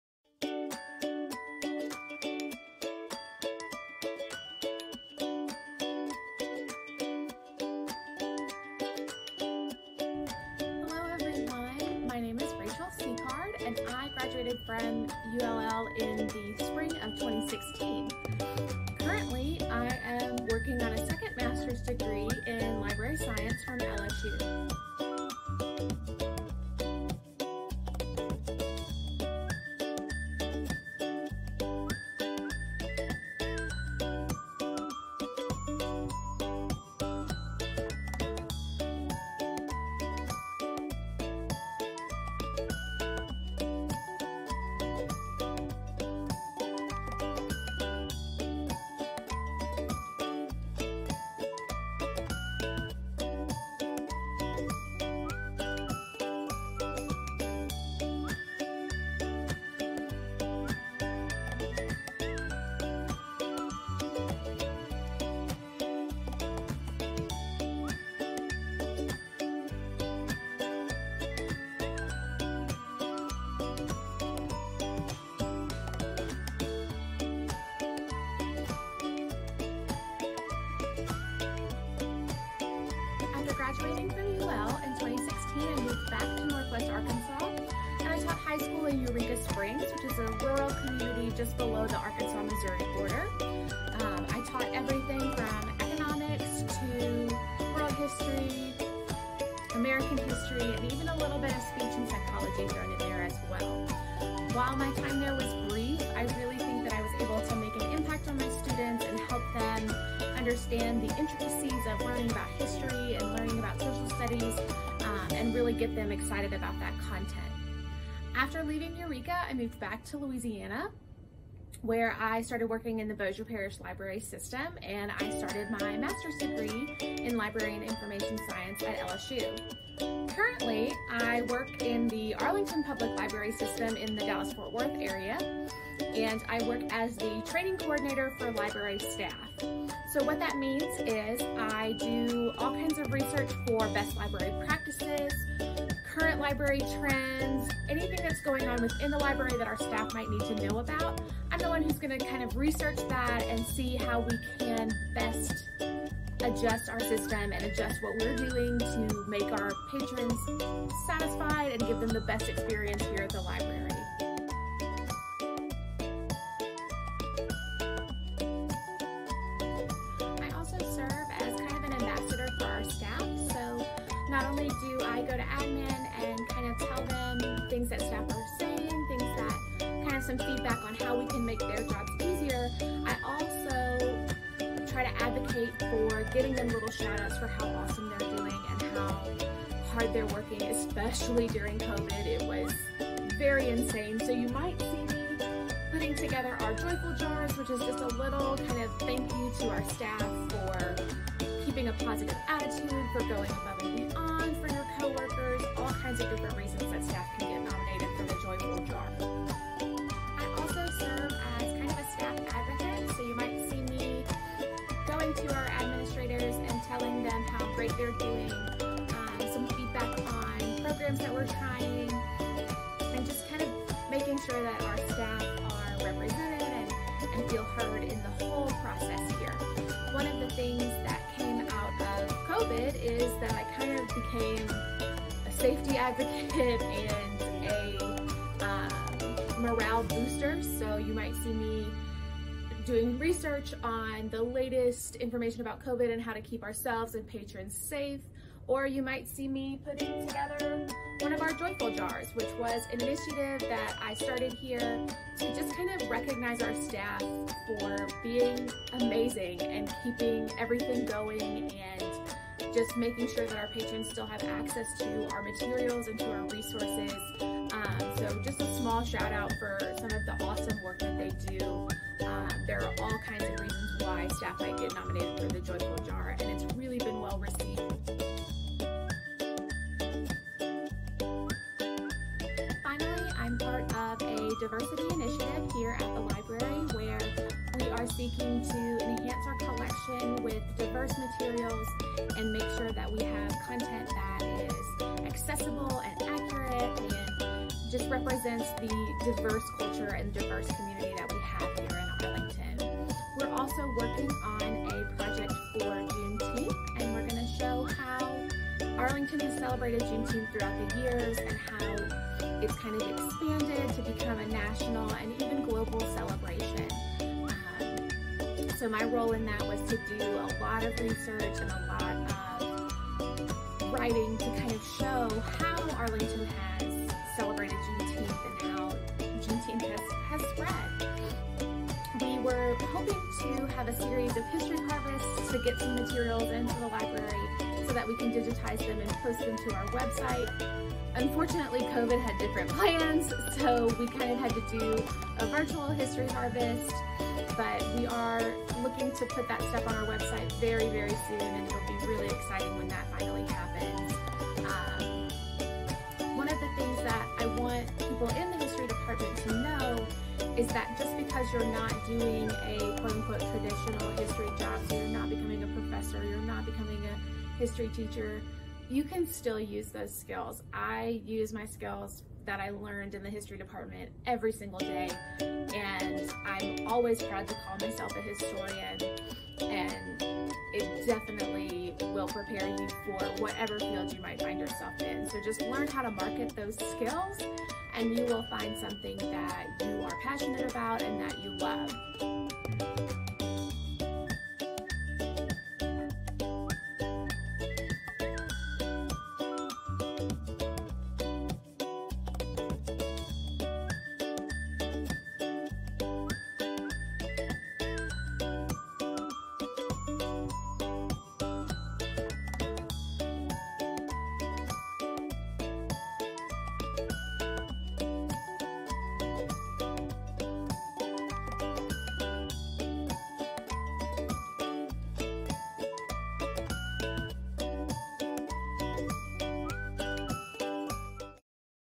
Alumni Spotlight-Rachel Sicard, Library Training Coordinator at Arlington Public Library
Join UL Lafayette Public History as we visit our alumni at their current jobs.

Meet Rachel Sicard, a 2016 graduate of the UL Lafayette MA Public History program, who is currently working towards her second master's degree in Library and Information Science from LSU. Rachel currently works for the Arlington Public Library, in Arlington, Texas as a Library Training Coordinator. She uses her public history degree to help her conduct research and vet resources for accuracy, relevance, and usefulness to the staff in her role as the Training Coordinator. Recently, Rachel assisted with the library’s Juneteenth community project to collect information and resources from their community to show and document the history of Juneteenth celebrations in Arlington, DFW, and beyond. As Rachel describes, “Initially, we hoped to host History Harvests for our patrons, but COVID-19 had other plans. We switched to individual scheduled ‘harvests’ with community partners and then opened an online submission forum where anyone could upload their materials.” The Arlington Black History Community Archive, as it is known, now includes historical photos, recordings, and documents that are free to the public and “honor, preserve, and communicate the rich heritage of Arlington’s Black community.” To read more about the Arlington Black History Community Archive, see the library’s